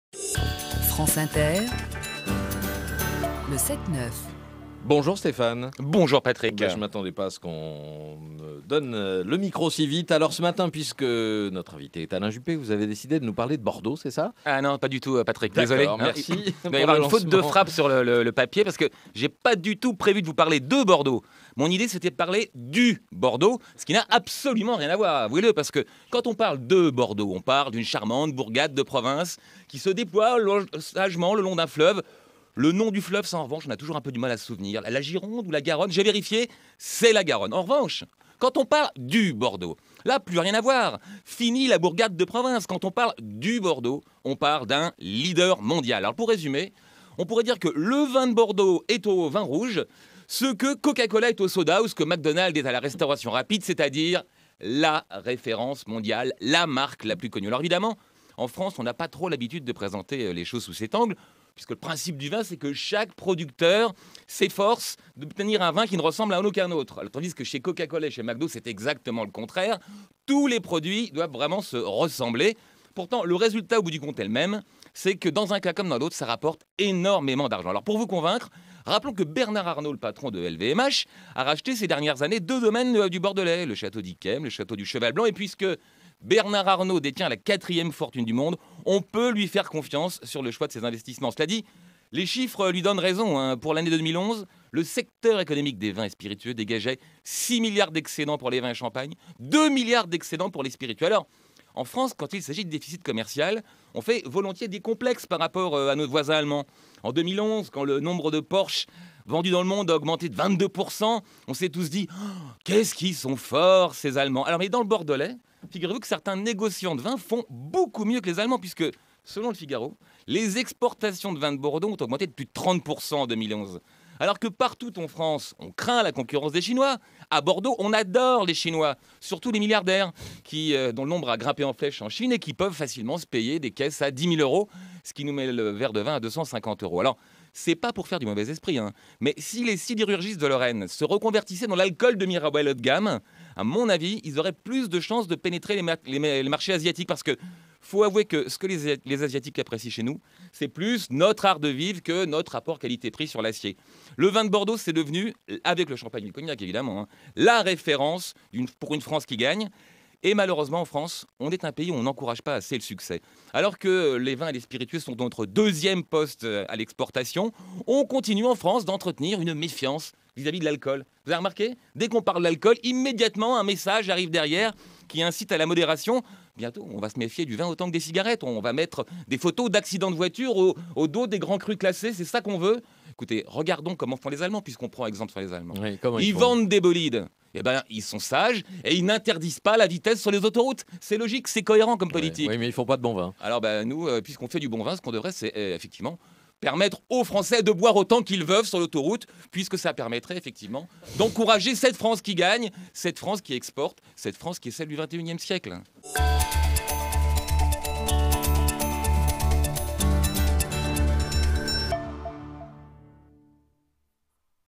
Le Bordeaux comme le Coca et le Mcdo - La chronique de Stéphane Blakowski
En France on a pas trop l'habitude de parler de nos produits phares exportés dans le monde entier. On complexe par rapport aux exportations de nos voisins allemands. Mais on oublie trop souvent notre succès mondial dans le vin grâce au vin de Bordeaux, véritable référence. Stéphane Blakowski plaide donc pour une reconnaissance et un soutien total à l'exportation de nos chers spiritueux nationaux (12 juin 2012 - 8h55).